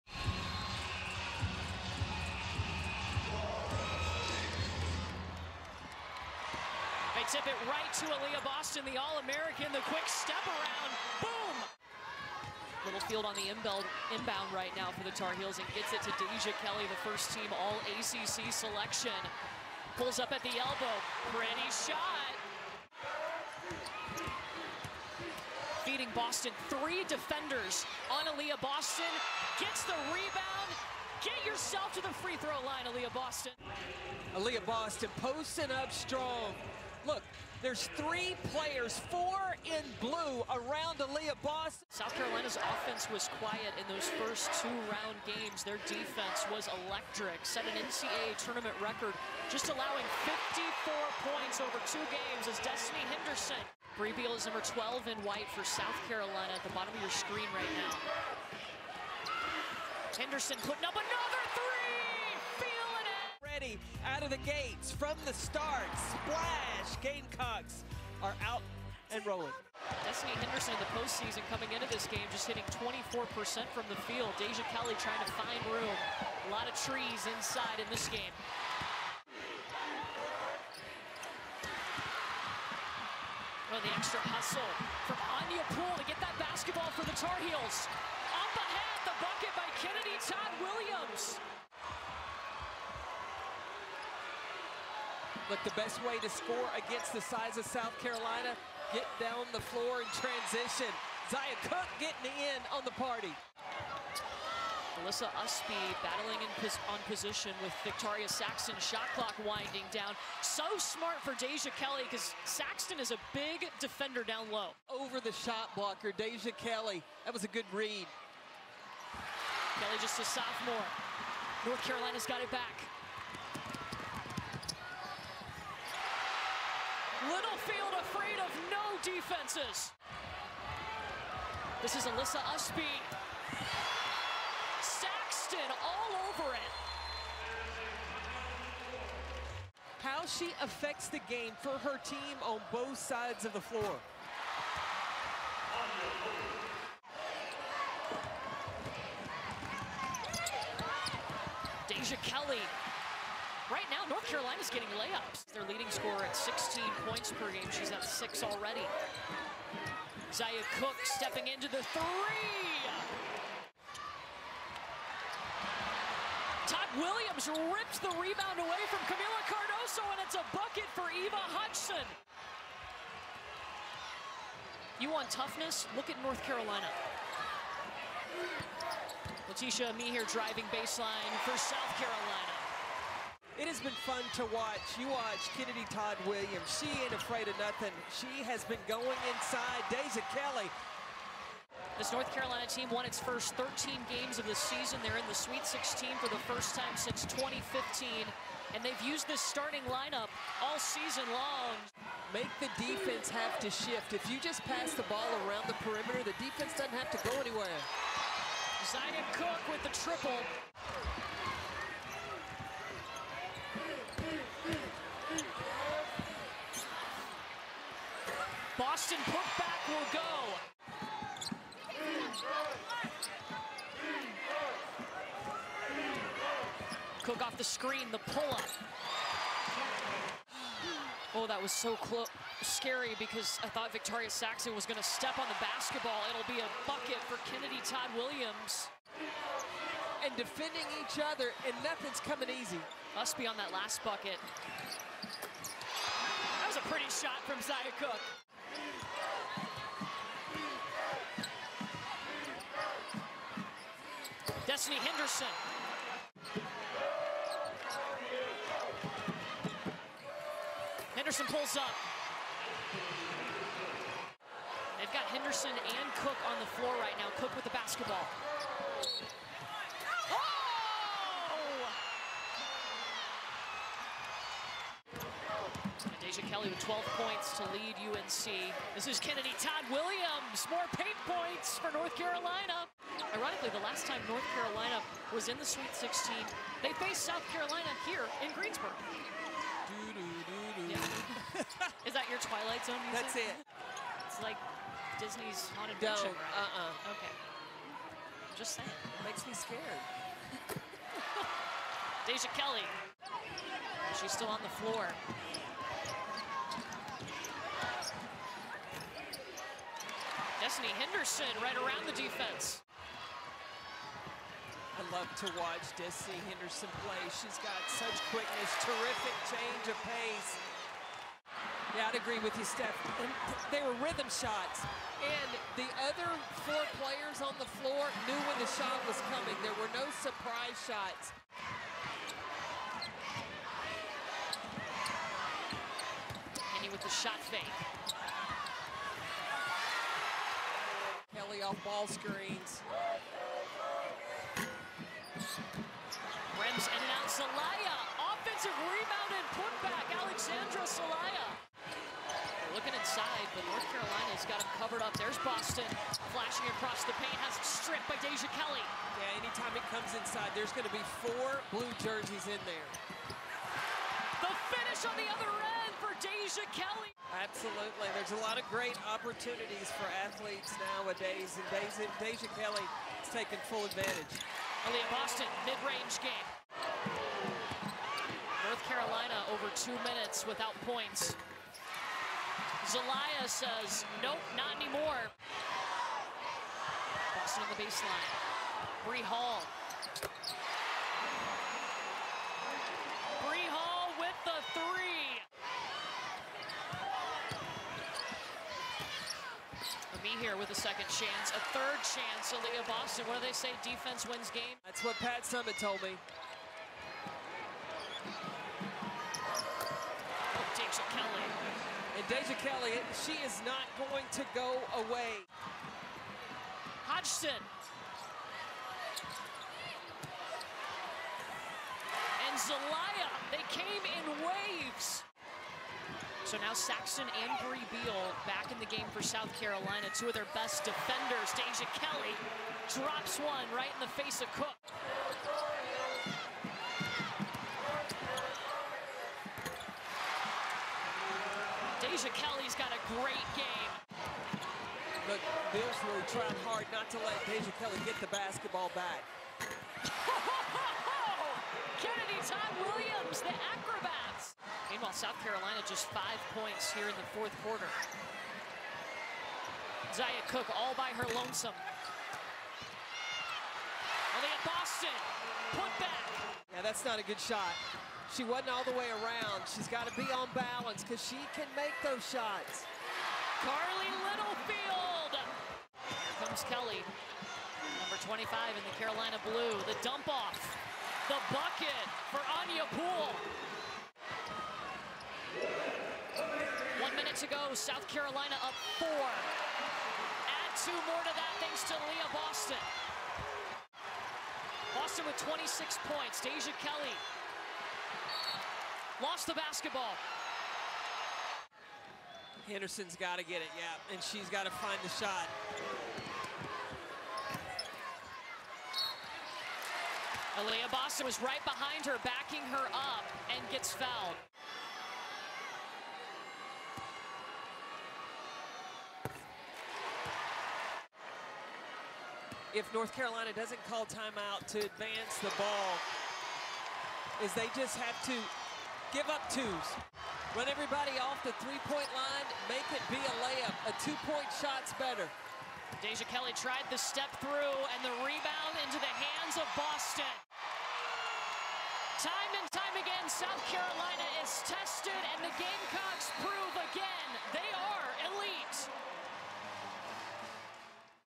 South Carolina vs. North Carolina - Women’s NCAA tournament Sweet 16 highlights
South Carolina survived a matchup against North Carolina, 69-61, in the Sweet 16 round of the 2022 women’s NCAA tournament. Naismith women’s player of the year finalist Aliyah Boston set an SEC record with her 27th straight double-double. Boston finished the game with 28 points and 22 rebounds. Watch the full extended highlights from South Carolina’s win here.